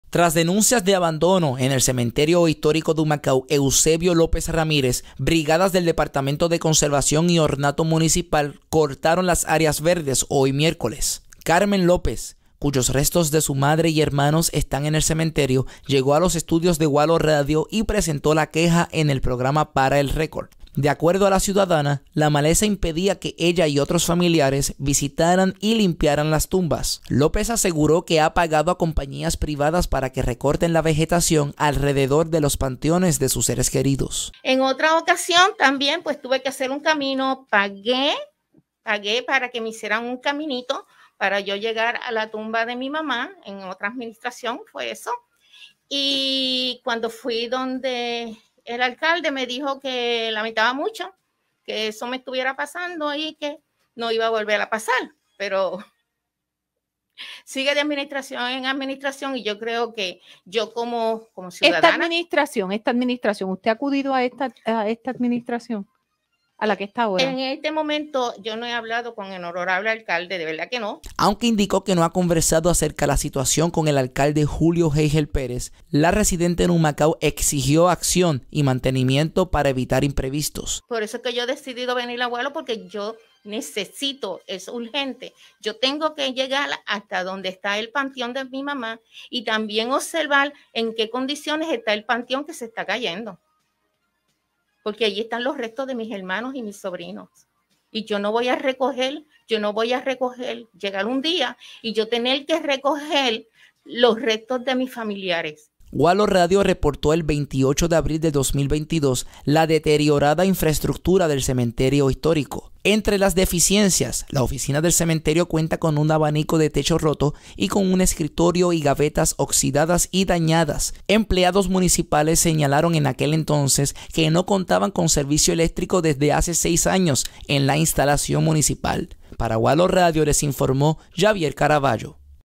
Brigadas municipales atienden denuncia de cementerio en Humacao | WALO Radio
(31-agosto-2022) Tras denuncias de abandono en el Cementerio Histórico de Humacao, Eusebio López Ramírez, brigadas del Departamento de Conservación y Ornato municipal cortaron las áreas verdes hoy, miércoles.

Karmen López, cuyos restos de su madre y hermanos están en el cementerio, llegó a los estudios de WALO Radio y presentó la queja en el programa Para El Récord.